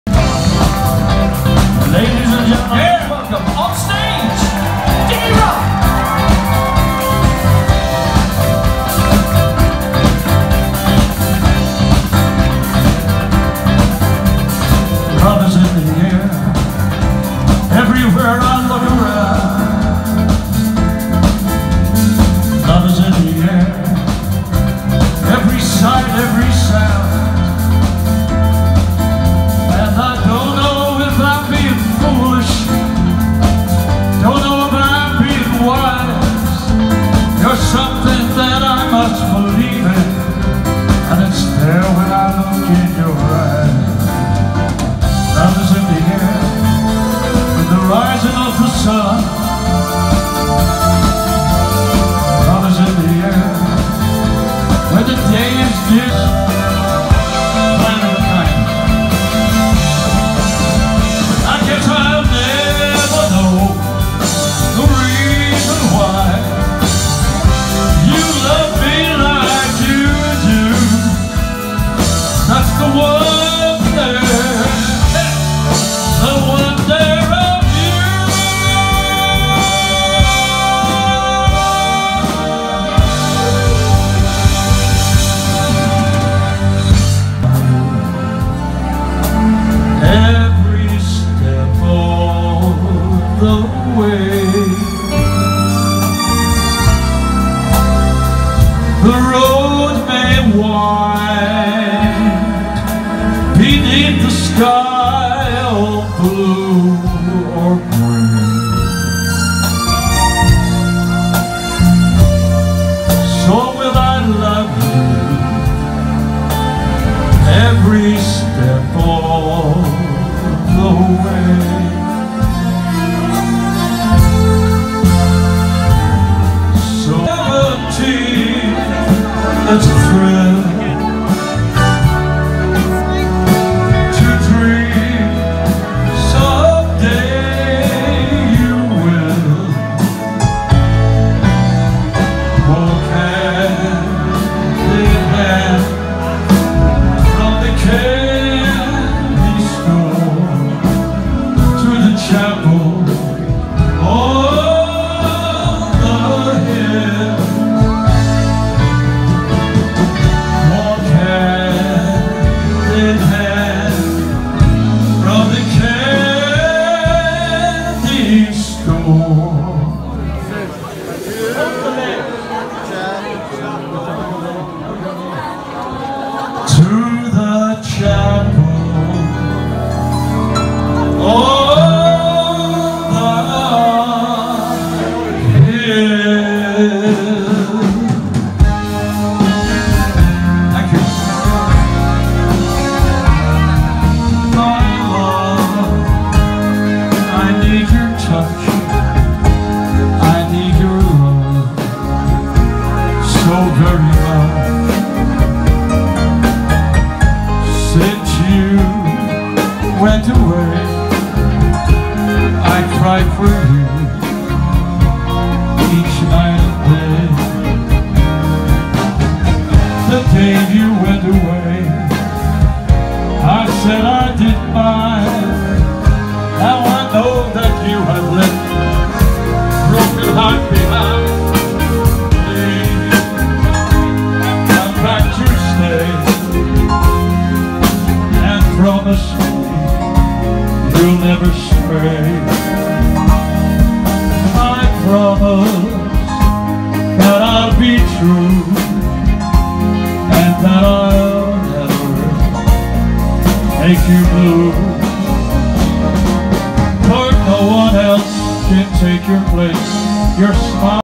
Dickie Rock Live in the Gleneagle Hotel Killarney 2019
Dickie Rock singing all his hits